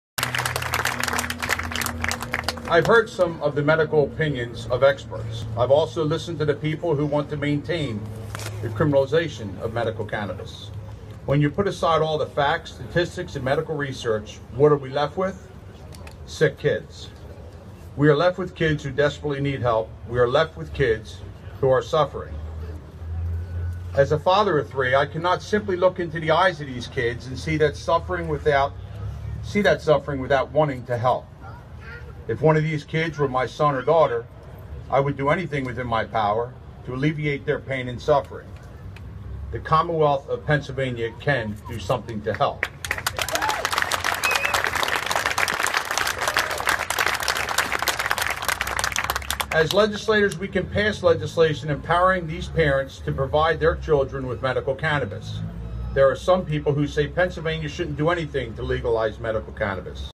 State Rep. Mike Vereb talks about changing his stance on medical marijuana.
By: Mark Walters - Runner, writer, Pennsylvanian.
Published on: September 15, 2014